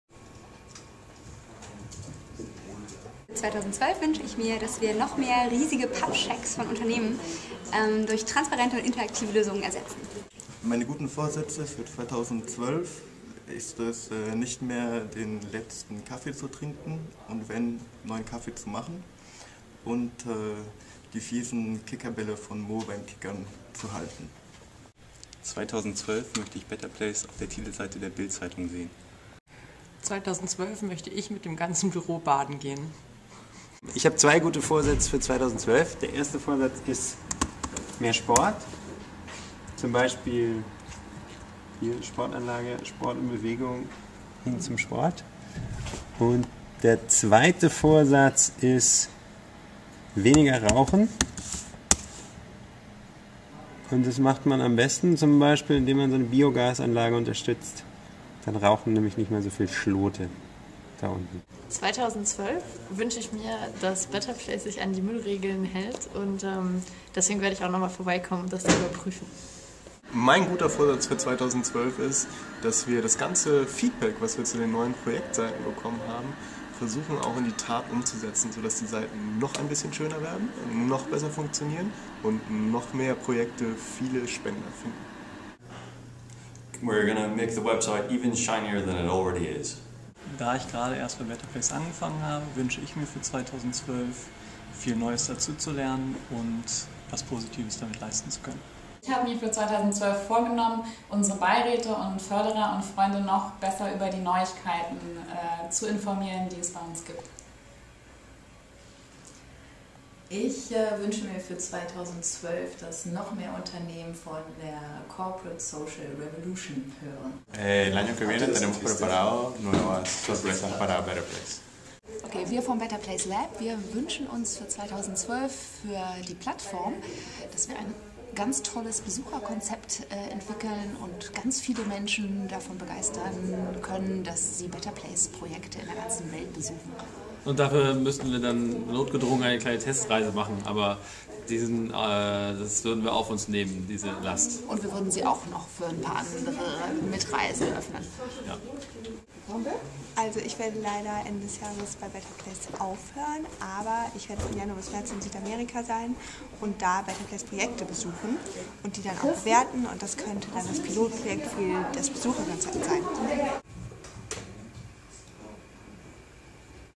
Gute Vorsätze 2012 bei betterplace.org
Im Jahr 2012 wollen wir das Weltverbessern natürlich weiter besser machen. Dabei haben einige aus unserem Team in einem spontanen Kurzinterview ihre guten Vorsätze fürs neue Jahr preisgegeben.